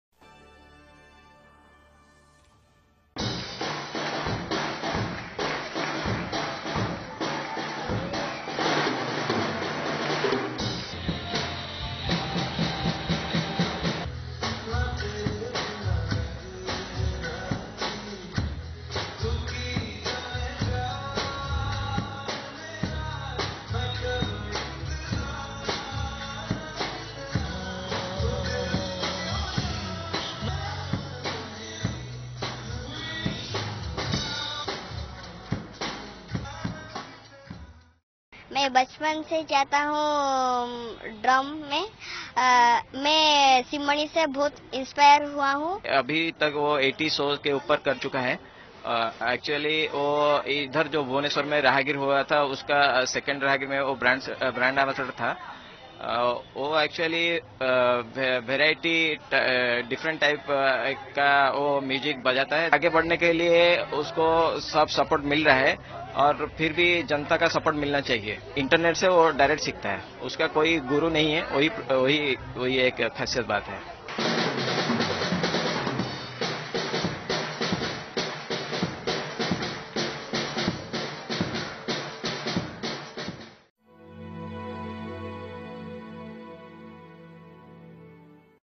9 Year Old Makes To "Britain's Got Talent" For Excellent Drumming Skills
A nine-year-old boy from Bhubaneswar has made it to "Britain's Got Talent" for his excellent drumming skills. A class-4 student, Abhishek Sahu, developed the passion for music when he got a drum at his home at the age of three. The child, who has 30 awards in his name, plays drum on Hindi, English, Odiya, Punjabi, Chinese and many other languages. Abhishek is set to fly to London on November 6.